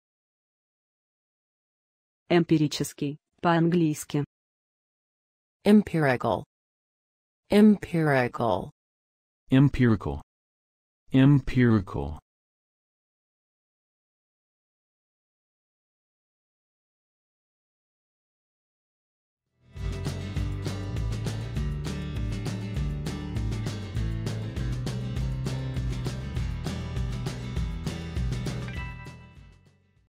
Эмпирический по-английски
Как правильно сказать Эмпирический по-английски? Слушай вместе с нами.
А как будет Эмпирический в переводе с русского на английский? Говори, как произносят американцы. И еще, как правильно написать слово Empirical?

Спасибо!